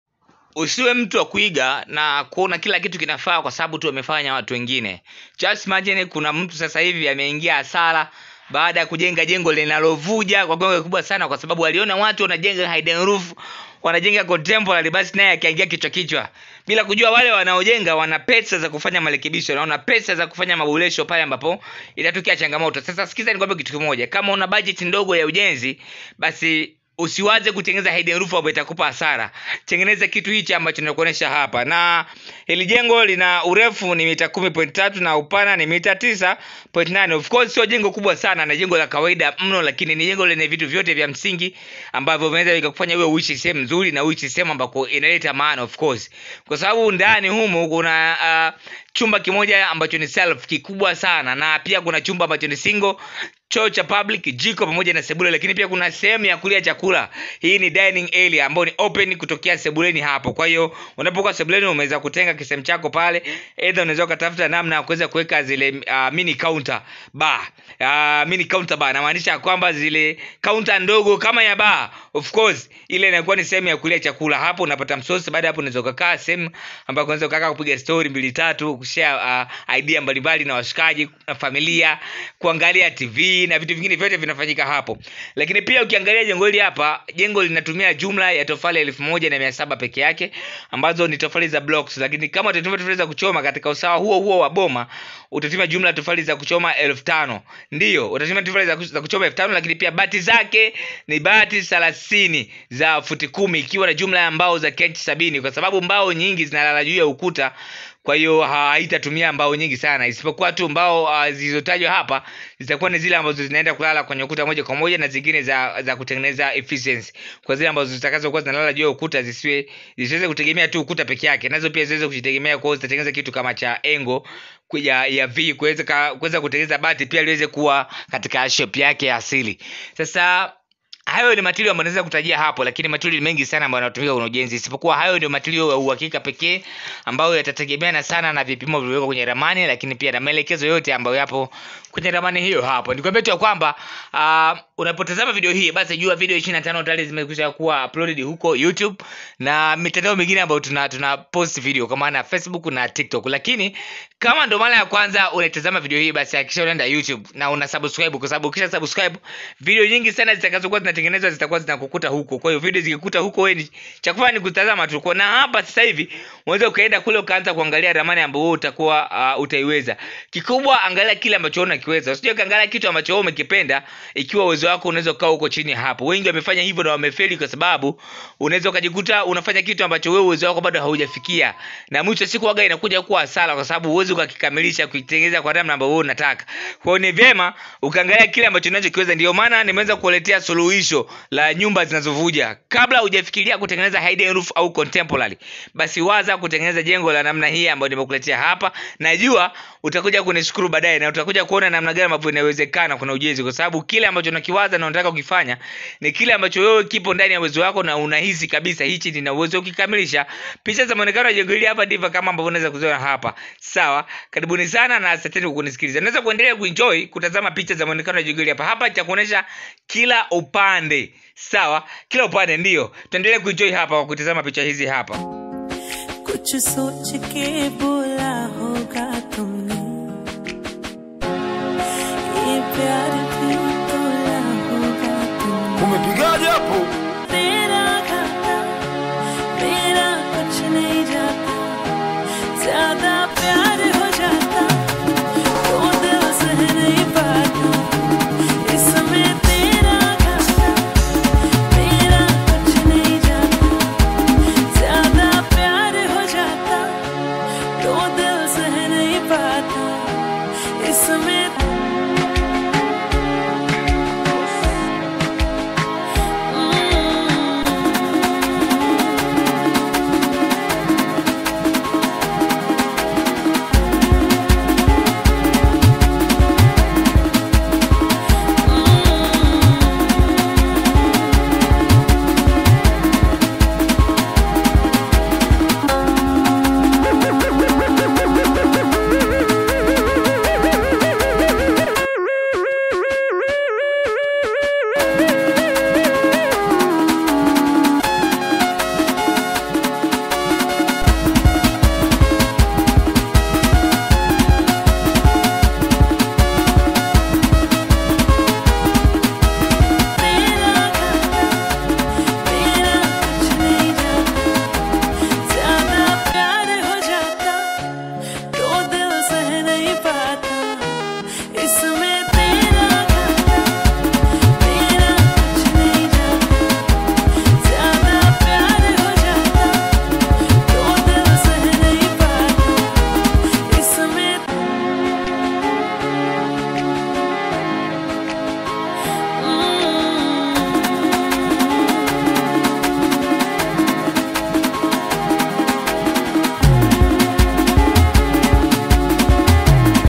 23. RAMANI NZURI SANA YENYE VYUMBA VIWILI VYA KULALA KIMOJA SELF
MPR/2BD/0071
RAMANI NZURI YENYE.
★VYUMBA VIWILI VYA KULALA.
(1, SELF, 1 SINGLE BEDROOM)
★SEBULE KUBWA,
★SEHEMU YA CHAKULA
★JIKO
★CHOO CHA WOTE.

KWA MAWASILIANO TUPIGIE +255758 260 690,

SASA UNAWEZA KUNUNUA KIWANJA KILICHOPIMWA MKOANI KATAVI KWA USALAMA WA ASILIMIA 100%